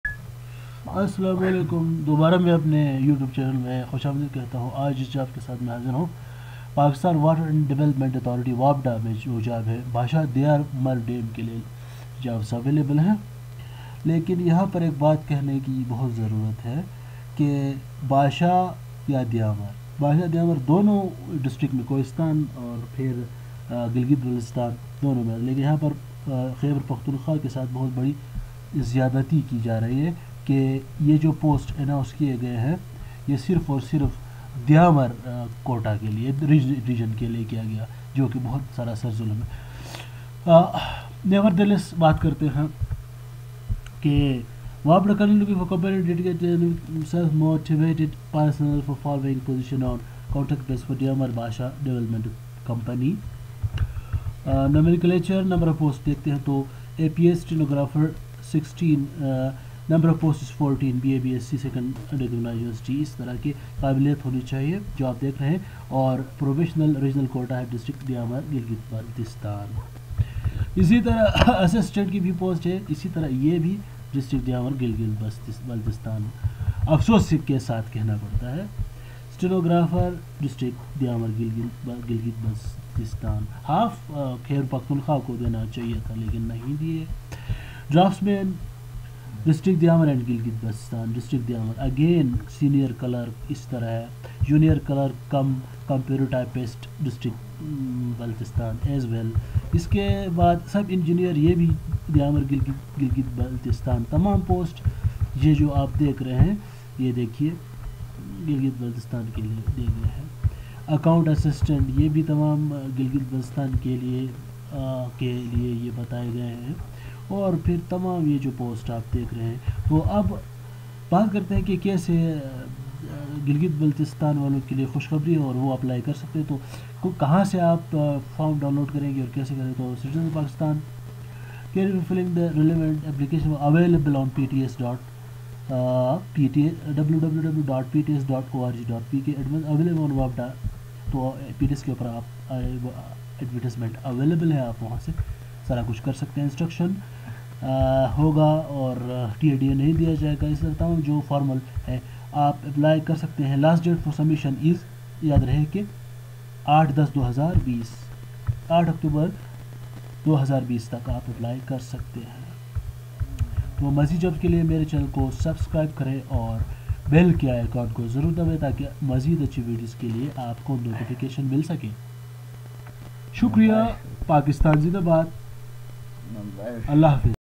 WAPDA Diamer Basha Dam Development Company (WAPDA-DBDC) new jobs apply hurry up
WAPDA is currently looking for competent, dedicated, dynamic and self-motivated personnel for the following positions apply
Contract basis for Diamer Basha Development Company: